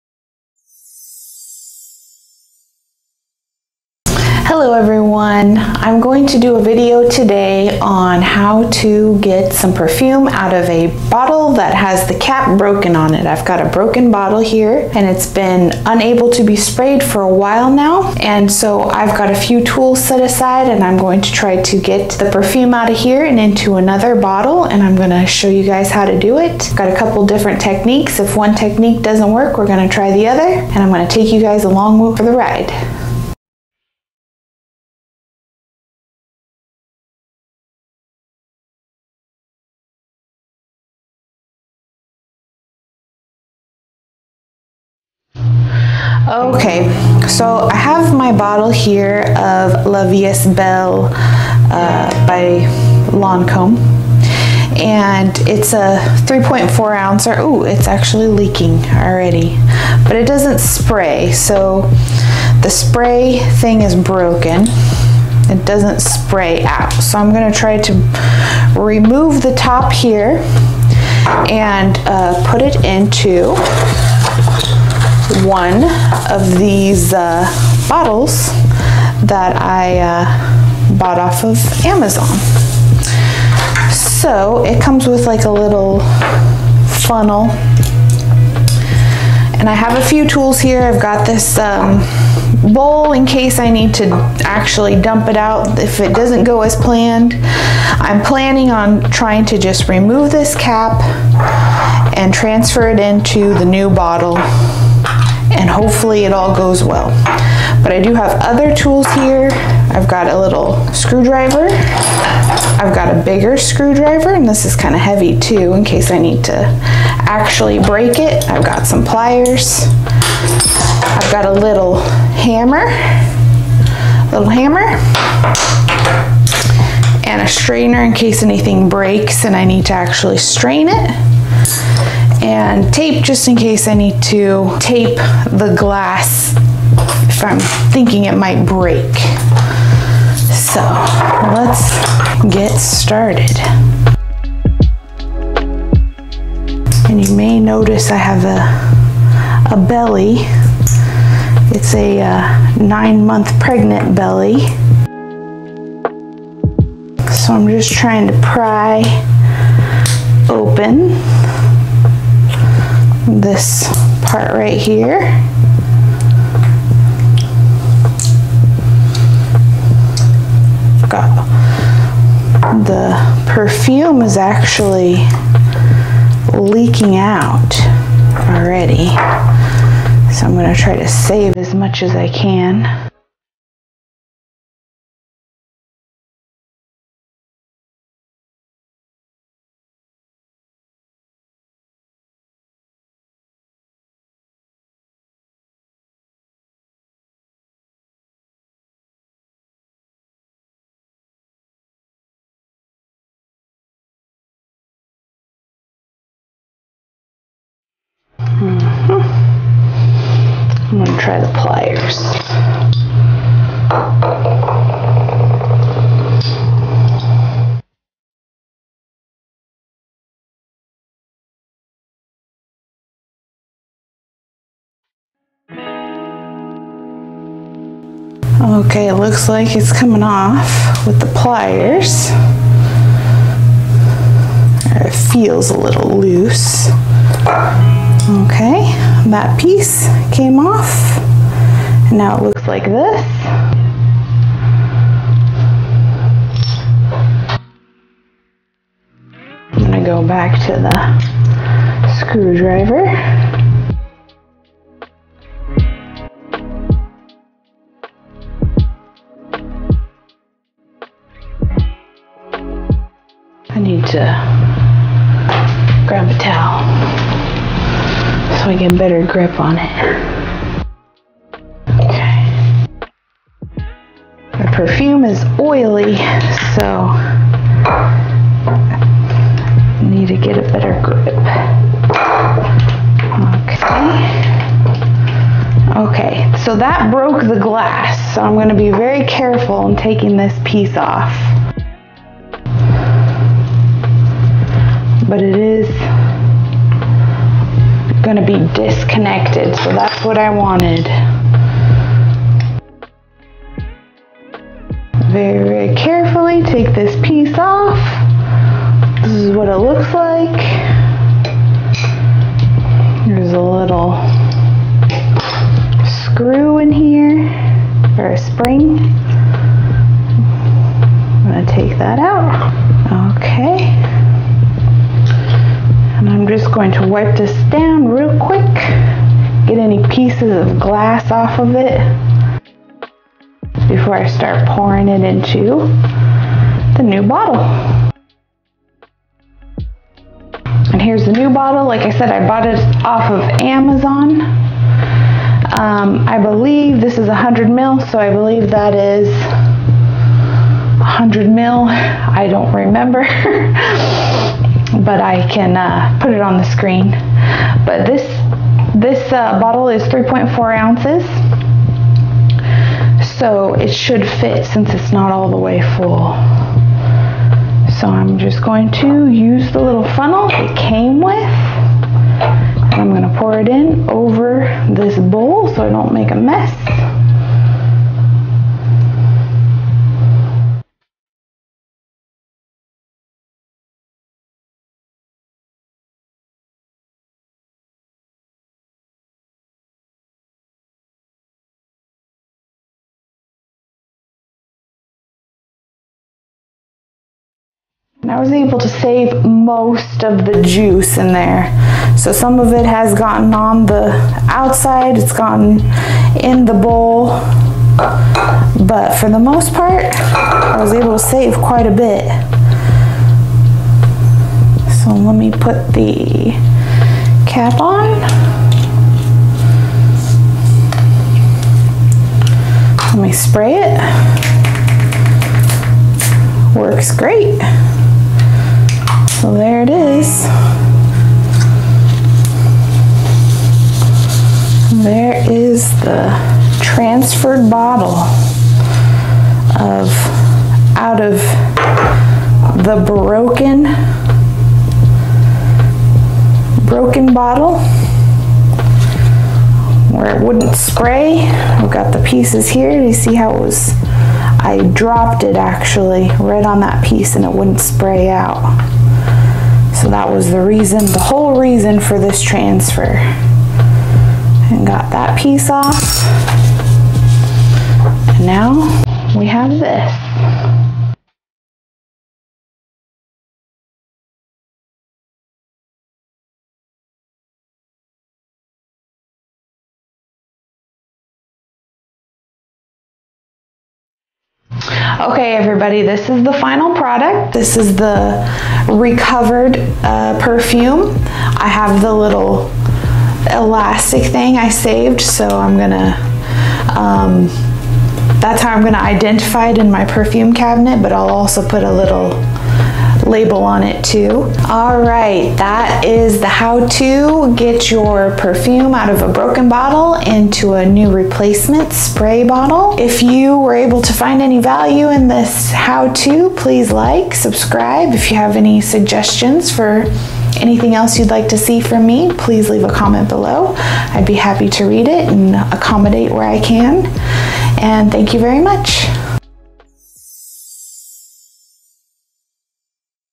How I replace a broken bottle of perfume
Hello everyone on today’s video I’ll be showing you how I replace a broken of perfume with a new bottle!